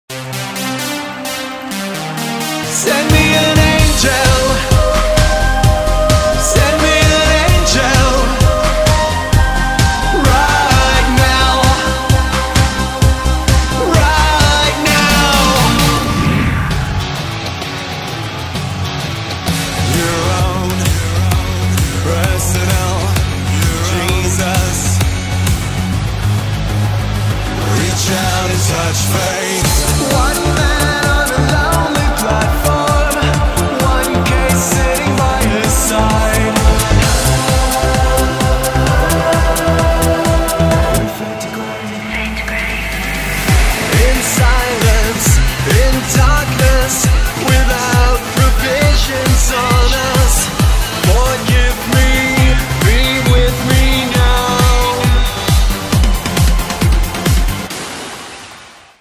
Christopher ANTON - Alphaville 2017 USA Tour opening
Christopher ANTON and the JONESES opening for Alphaville USA 2017  Tour!